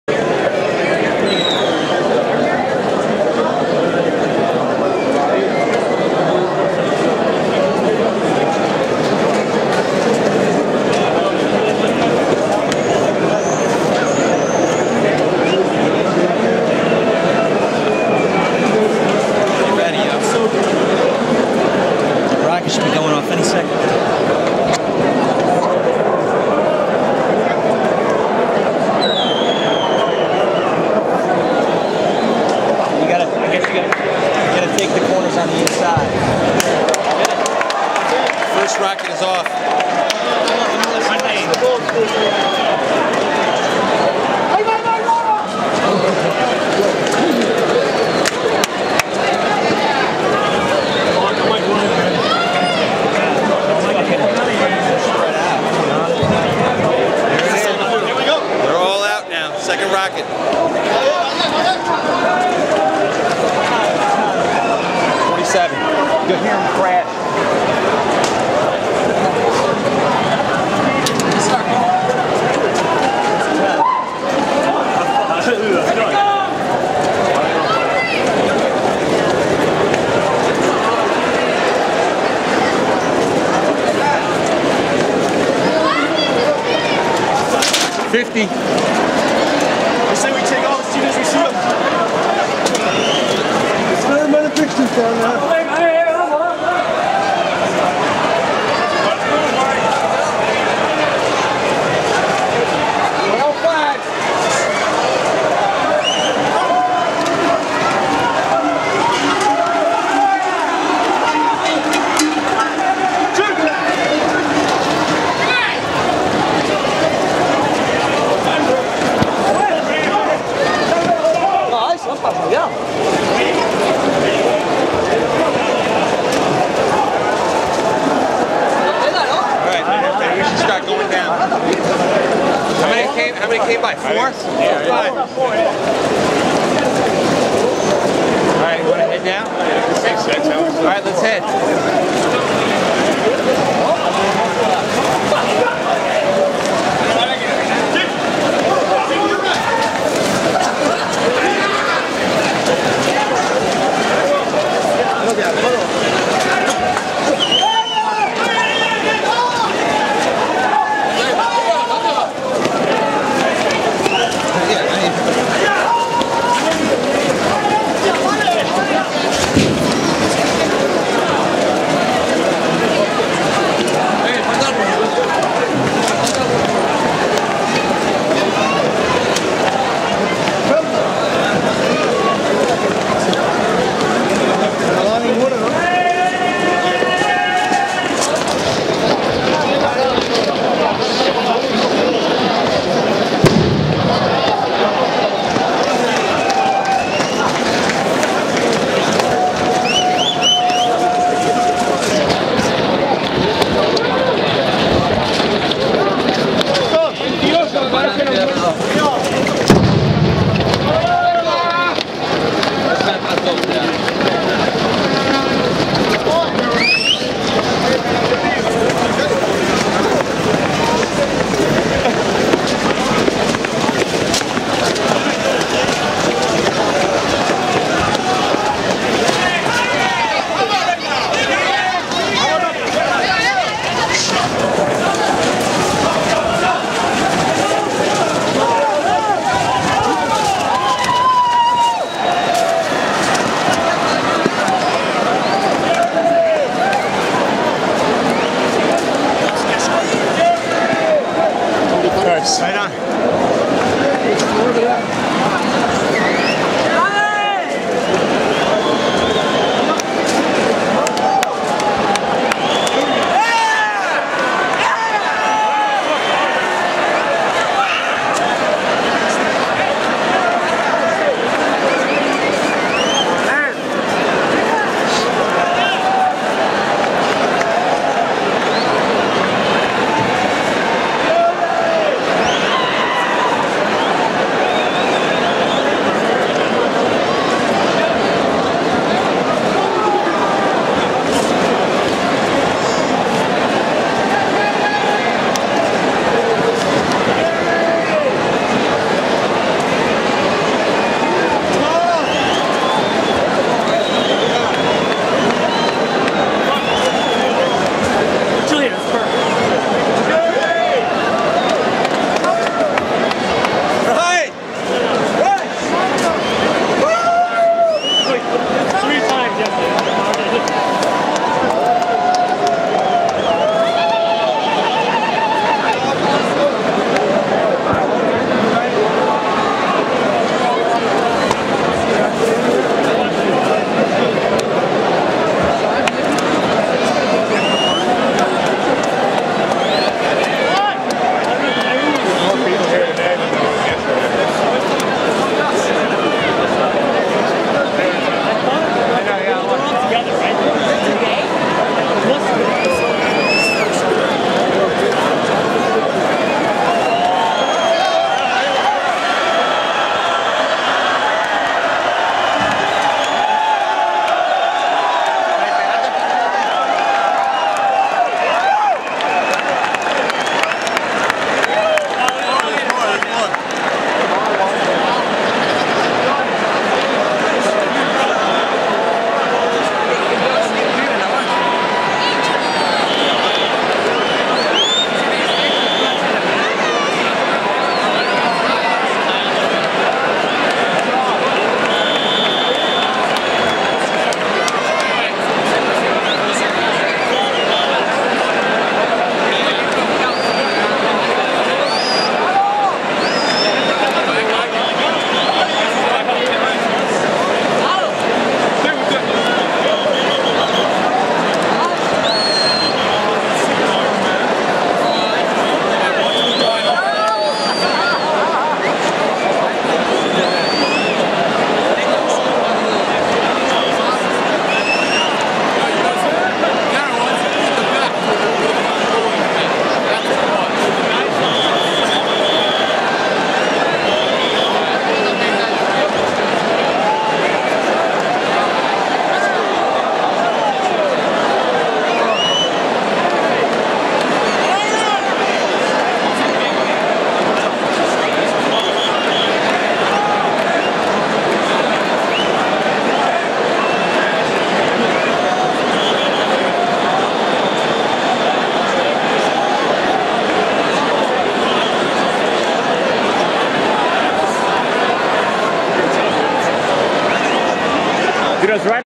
Running of the Bulls in Pamplona, Spain 2008 July 8
The bulls run right past us on the way to the ring!